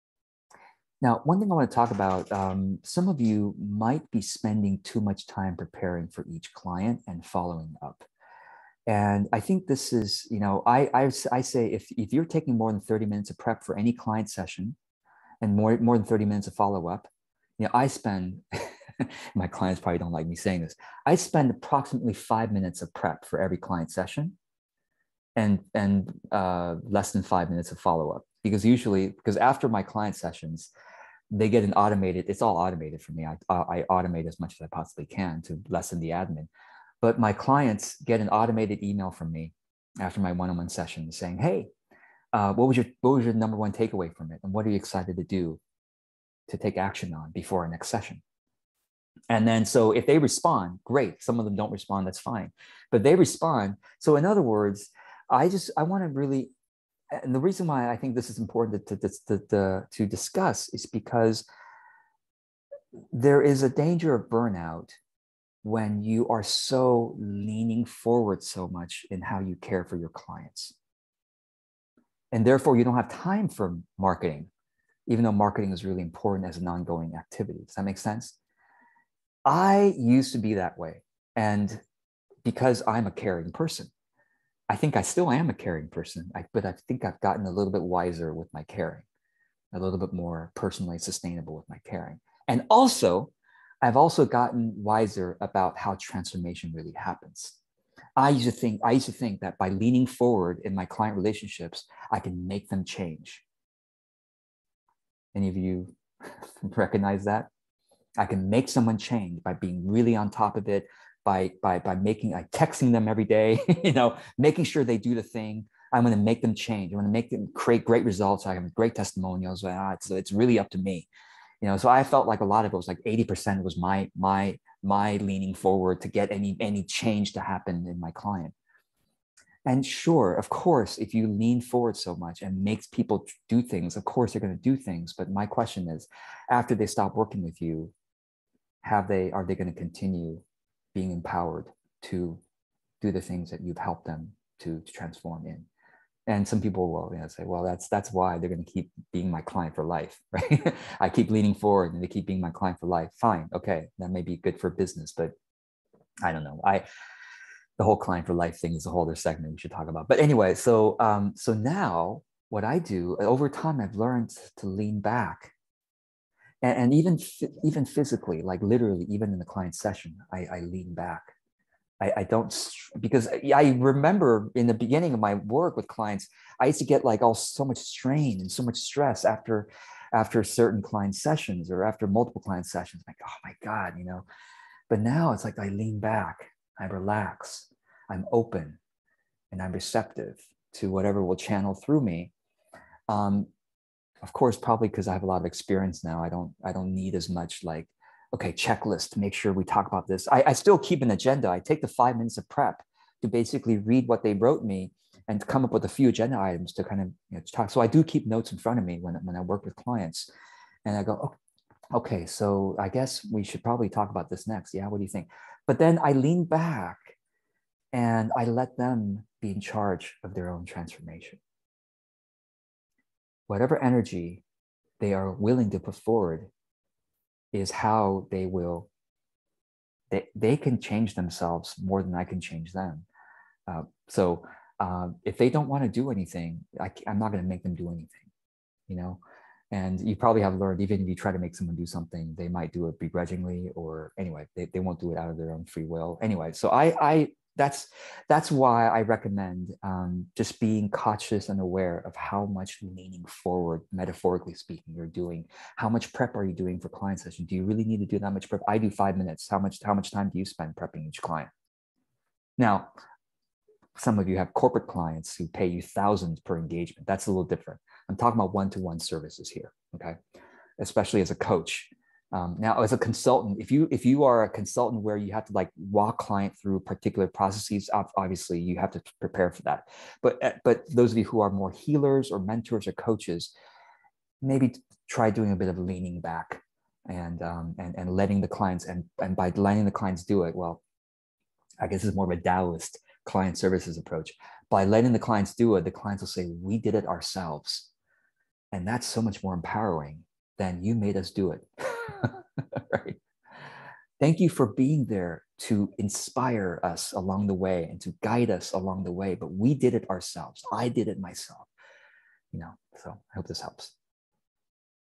Spending lots of time & energy prepping clients? "Lean back." Let them lead their own transformation
This is a segment from my Monthly Q&A call. You get access to 2 months of these live calls when you sign up for any of my courses

MonthlyQA